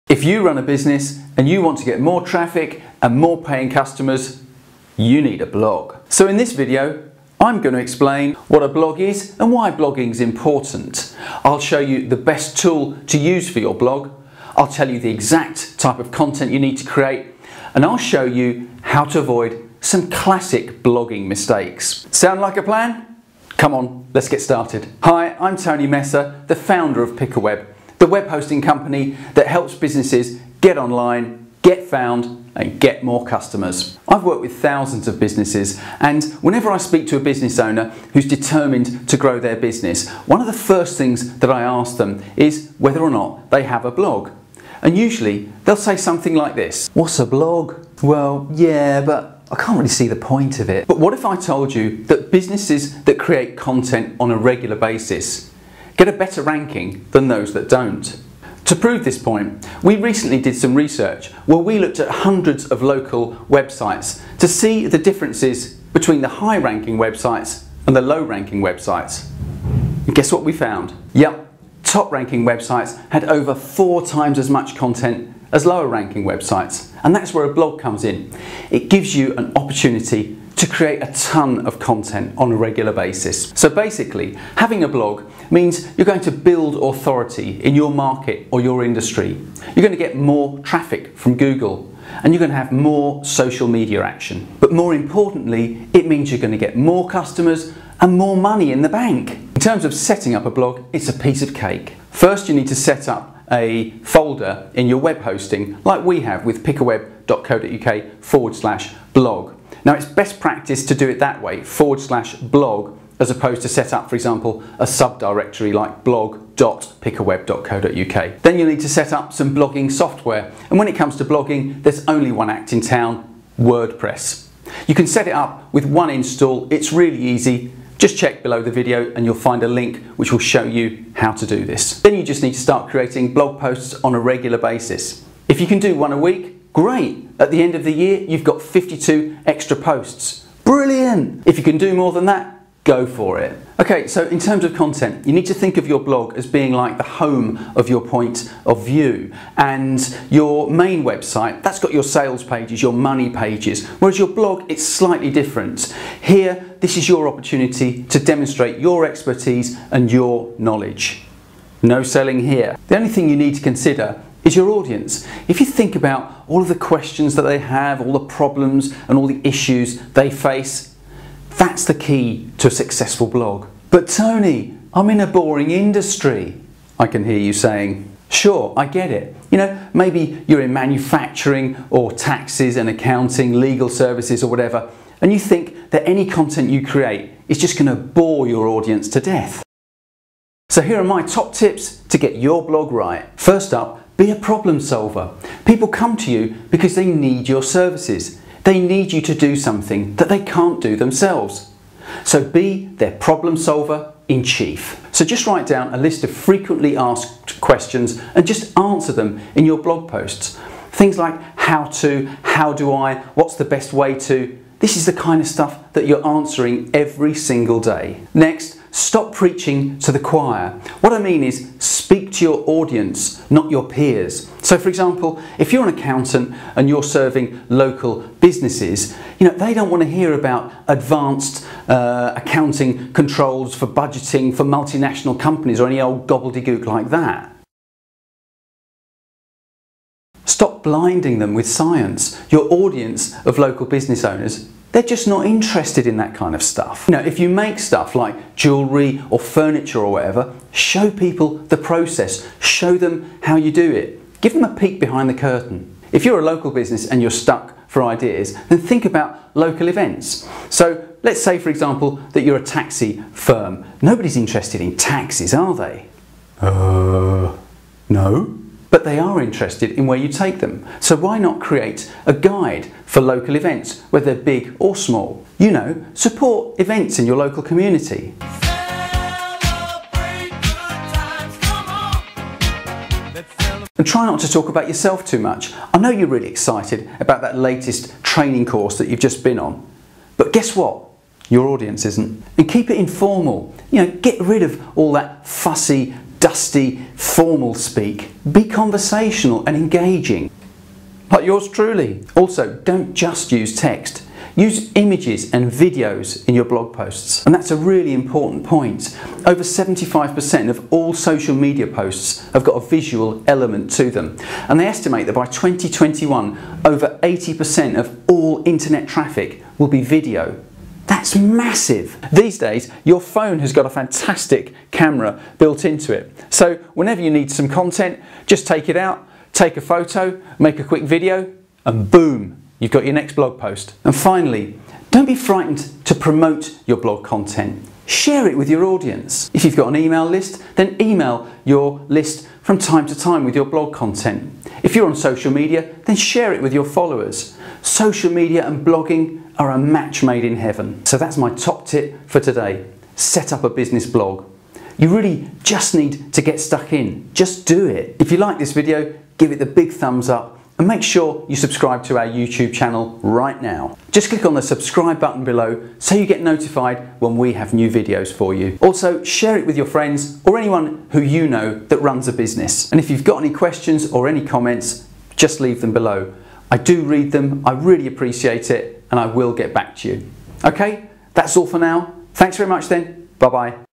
How To Start a Blog - Step By Step - Tony Messer Explains!
Tony Messer explains How To Start a Blog step by step.

If you run a business and you want to get more traffic and more paying customers then you need to create a blog.

So in this video I’m going to explain how to start a blog step by step.

What a blog is & why it’s important
I’ll show you the best tool to create your blog
I’ll tell you the exact content you need to create
& I’ll show you how to avoid some classic blogging mistakes

Hi, I’m Tony Messer - the founder of Pickaweb , the web hosting company that helps businesses get online, get found and get more customers.

For more details about this video check our blog post How To Start A Blog - Beginners Guide Business Blogging